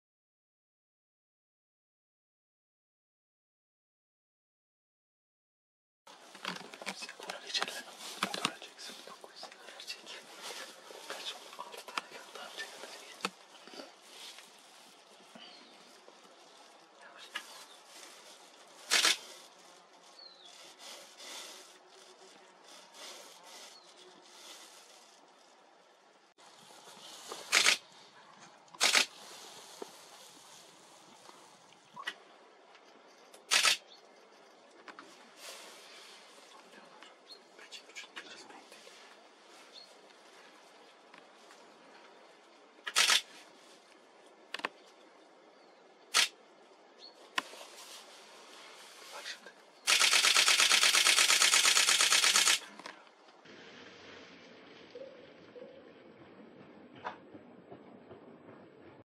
Kurtlara yem oldu!
Afyonkarahisar'ın Sandıklı ilçesinde kurt sürüsünün beslenmesi görüntülendi
AYRINTILAR ODAK'TA...